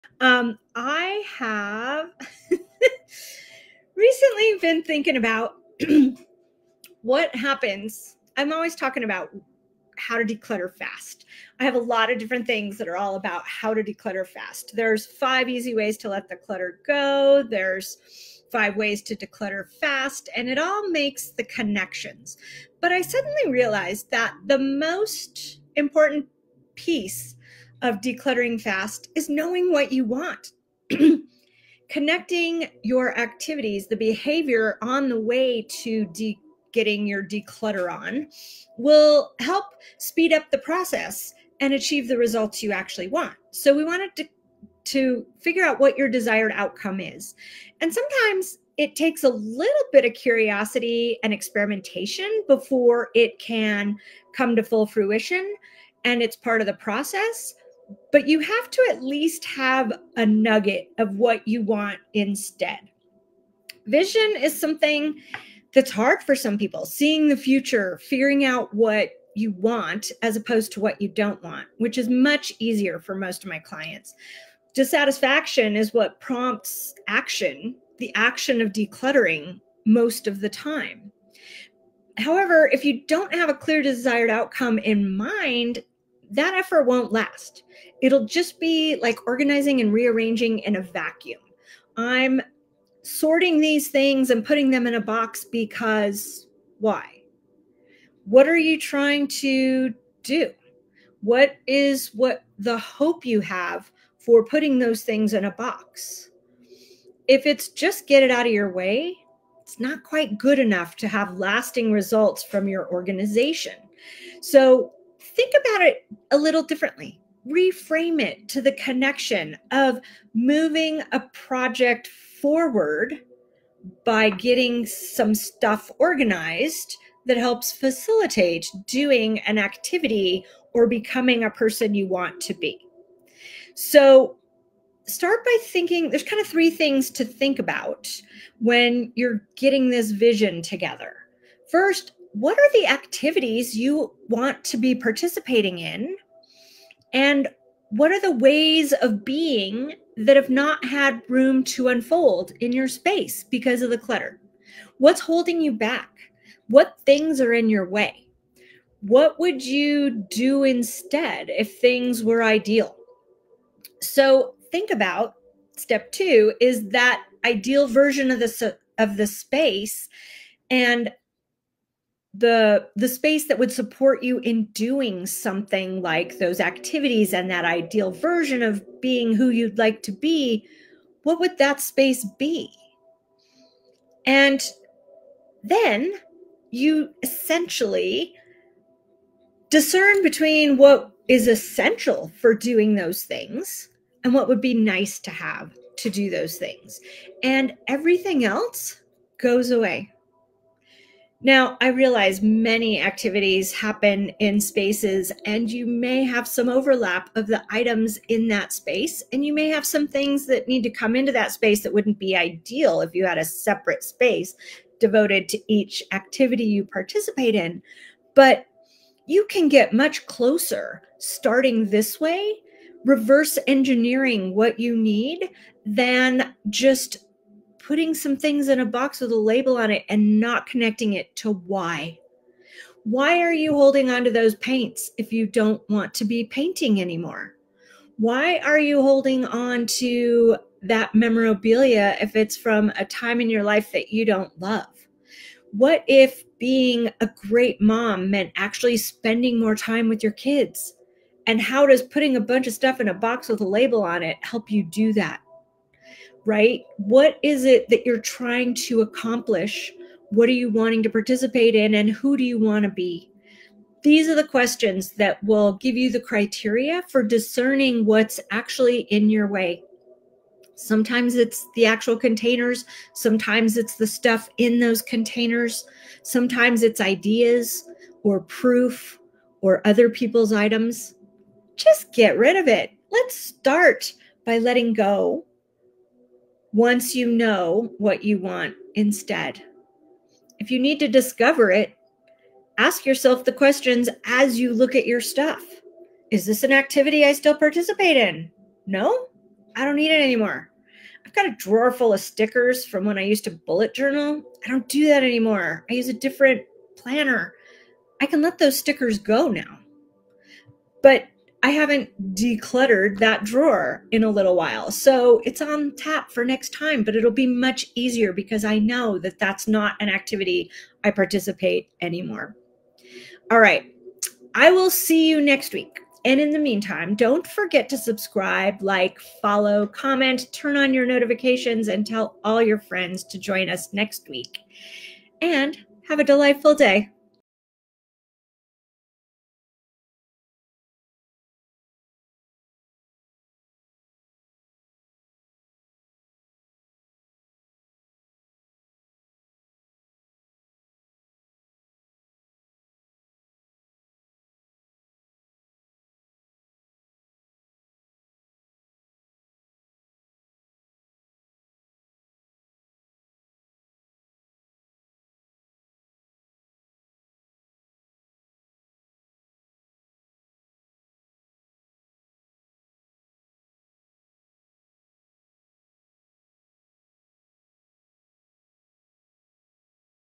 The Key to Lasting Decluttering: It's Not What You Think (But It Works!) | More than Organized
Struggling to declutter and keep things organized? Tired of the same old "tidy up" tips that never seem to stick? In this video, we reveal the game-changing secret to decluttering success: having a clear desired outcome.

Most decluttering advice focuses on speed and ease, but what if the key was knowing WHY you're decluttering in the first place?  We'll show you a simple 3-step process to connect your decluttering efforts to your goals and aspirations.

Stop feeling overwhelmed and frustrated! Learn how to declutter with purpose and create a space that truly supports the life you want to live.

In this video, you'll discover:

❓ Why traditional decluttering methods often fail
🧠 The powerful mindset shift that will transform your decluttering journey
🎯 A step-by-step process to identify your ideal outcome
✅ How to use your desired outcome to make decluttering decisions with ease
👍 Practical tips to declutter faster and achieve lasting results

Ready to finally ditch the clutter and create a space that inspires you?

Watch now!